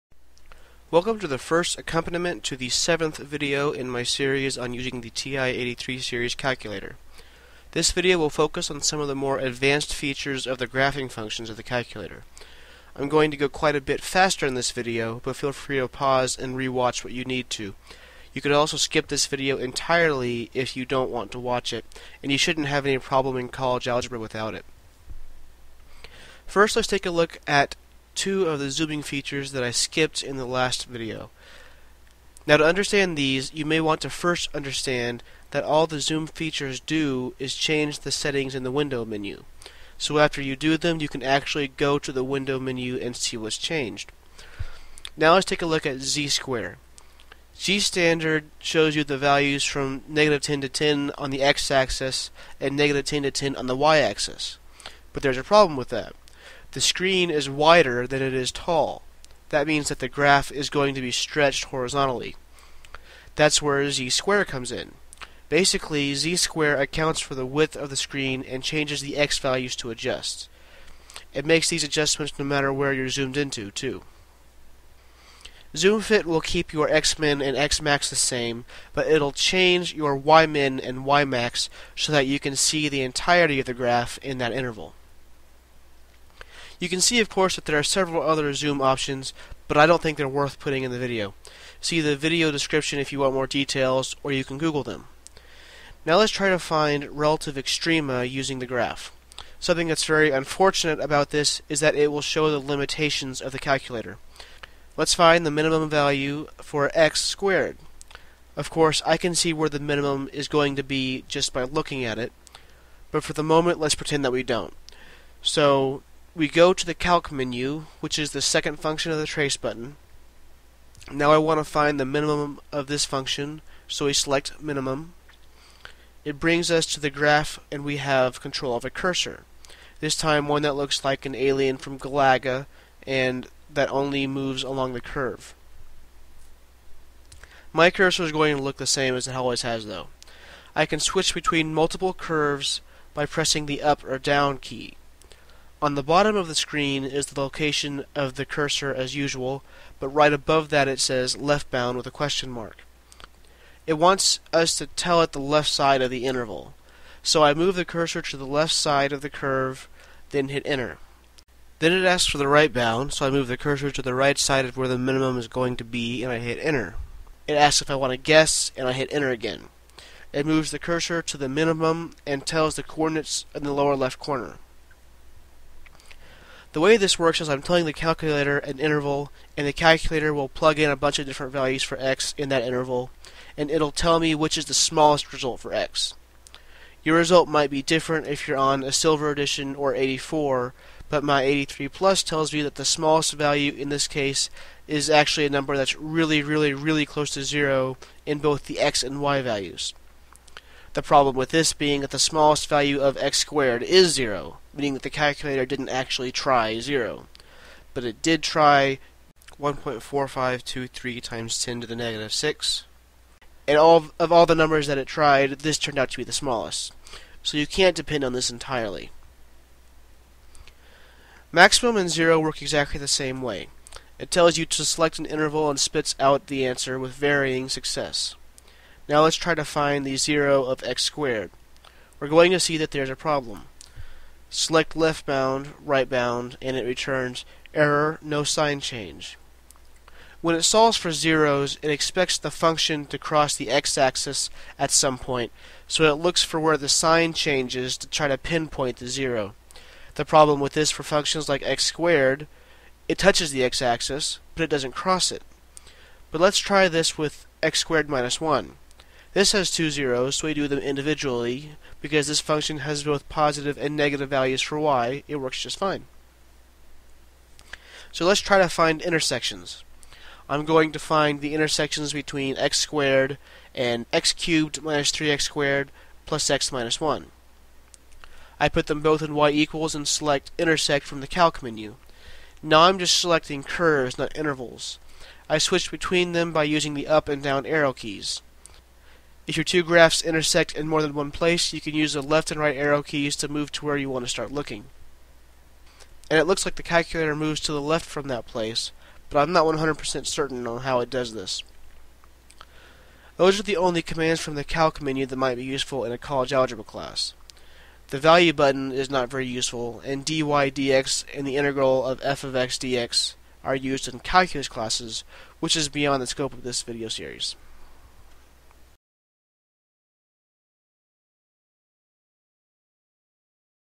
Using the TI-83/84: 7.1 - More Zooming, Finding Relative Extrema, Zeros, and Intersections
This is the first accompaniment to the seventh part of my series on using the TI-83 series calculator.  This video focuses on more of the zoom features as well as the functions in the Calc menu, being finding zeros, finding minimum and maximum, and finding intersections.  This is an advanced video, and you can skip it if you don't want to watch it.  This one moves pretty fast compared to the other videos, so be prepared to pause and rewatch parts if you need to.

Regarding ZDecimal and ZInteger:  You probably noticed before that the coordinates of the cursor have funky numbers, like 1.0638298 instead of just 1.  ZDecimal is one way to fix that.  It changes the cursor to only move to .1, .2, etc.  ZInteger is also another way to fix that.  It changes the cursor to only move to integers.  ZInteger actually requires that you hit enter while you're on the graph after selecting it.

ZTrig is something that you'll use in Trigonometry.  It changes the tick marks on the X axis to correspond to 90 degrees, 180 degrees, 270 degrees, 360 degrees, and so on.  This will be different in radian mode, where it'll switch the tick marks to pi/2, pi, 3pi/2, 2pi, and so on.  So you definitely want to set your mode before you hit ZTrig, because if you hit ZTrig, then switch from radians to degrees or vice versa, it'll do something funky.  You can fix it by just hitting ZTrig again.

ZoomStat does the same thing as ZoomFit, but for plots instead of functions.  We'll get to plots in a later video.